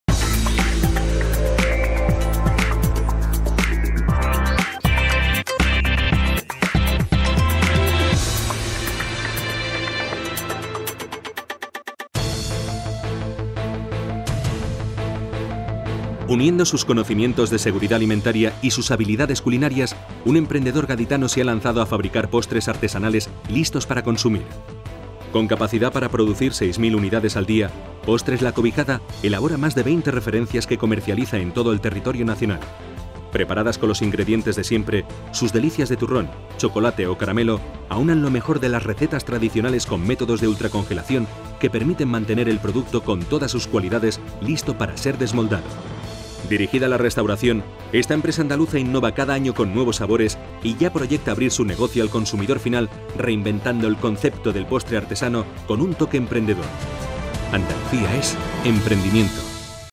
HDL Postres La Cobijada - Emprendimiento
Uniendo sus conocimientos de seguridad alimentaria y sus habilidades culinarias, un emprendedor gaditano se ha lanzado a fabricar postres artesanales listos para consumir.
Con capacidad para producir seis mil unidades al día, Postres La Cobijada elabora más de veinte referencias que comercializa en todo el territorio nacional. Preparadas con los ingredientes de siempre, sus delicias de turrón, chocolate o caramelo aúnan lo mejor de las recetas tradicionales con métodos de ultracongelación que permiten mantener el producto con todas sus cualidades listo para ser desmoldado.
Dirigida a la restauración, esta empresa andaluza innova cada año con nuevos sabores y ya proyecta abrir su negocio al consumidor final reinventando el concepto del postre artesano con un toque emprendedor.